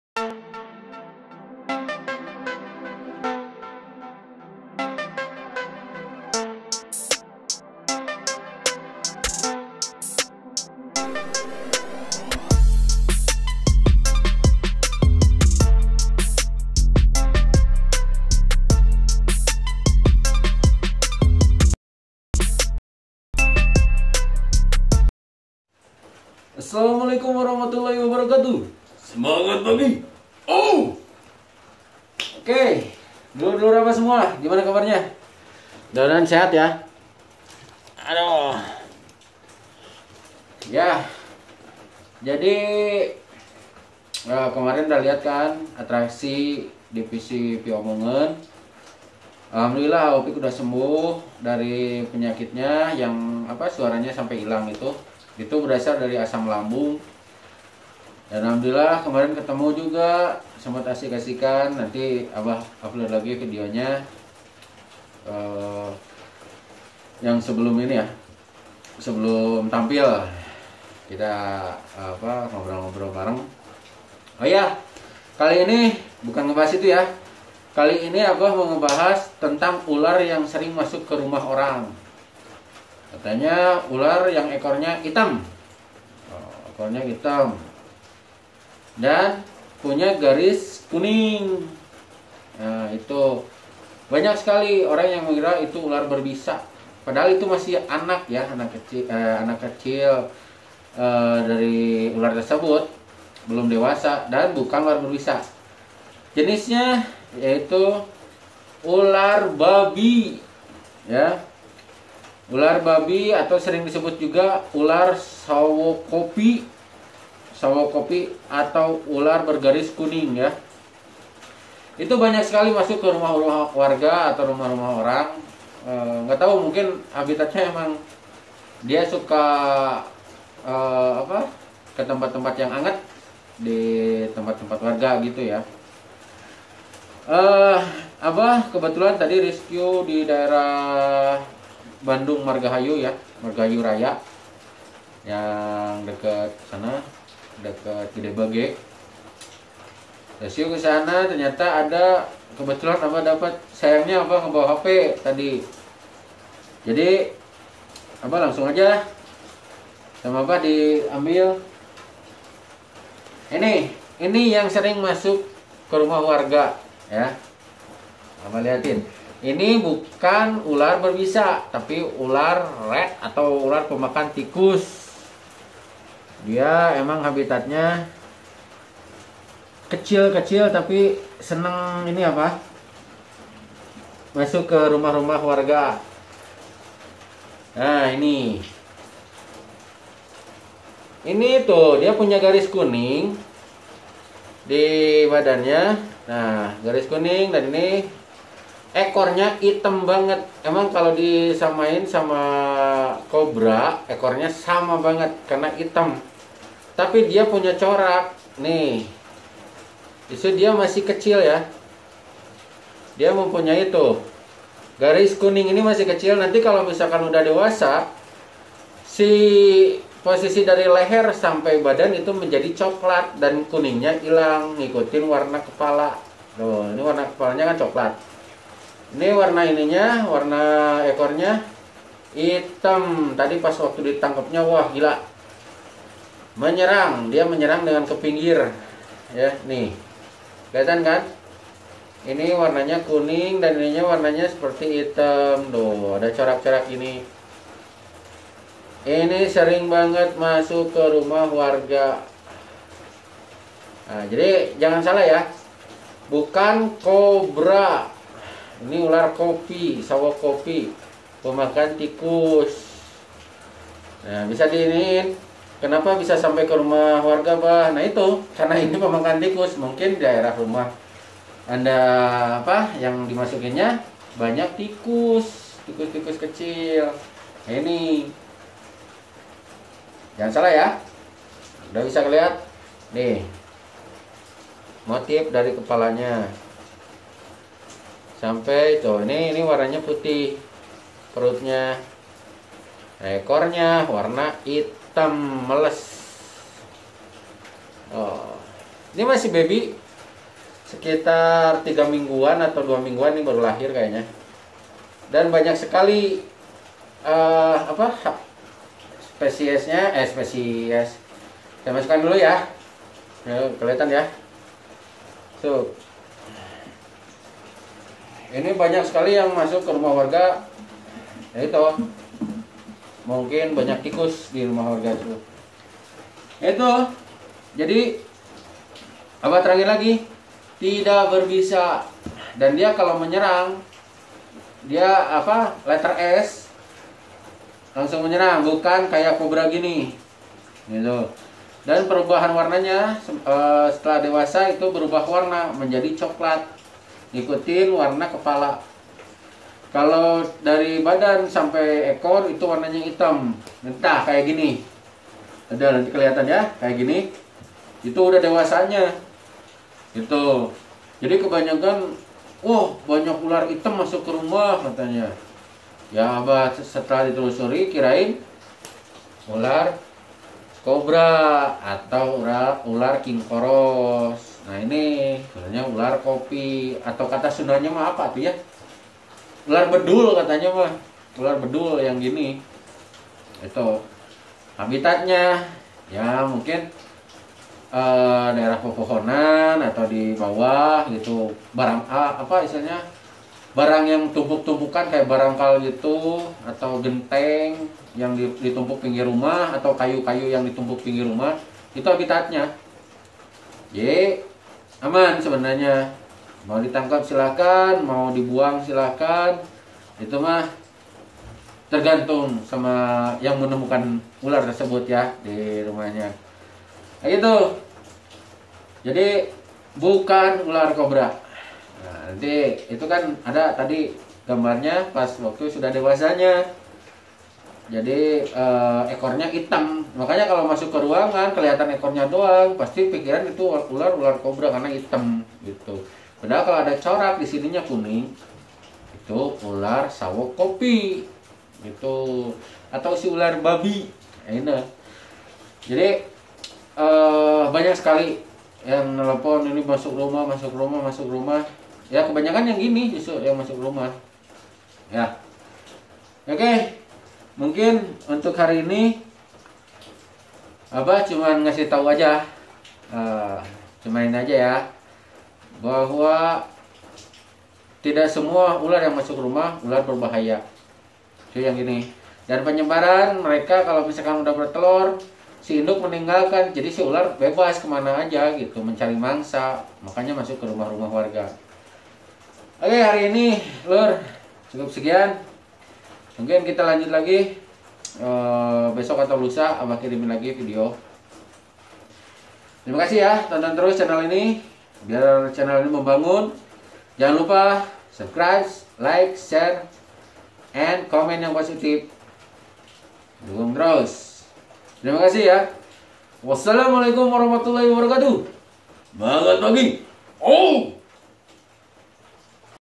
ular yg sering masuk rumah //
banyak sekali warga yg mengeluh banyaknya ular masuk ke pemukiman warga dan banyak se x yg tidak tau itu jenis ular apa ada yg berbahaya adapun yg tidak berbahaya tp kebanyak ular yg masuk ular yg tidak berbahaya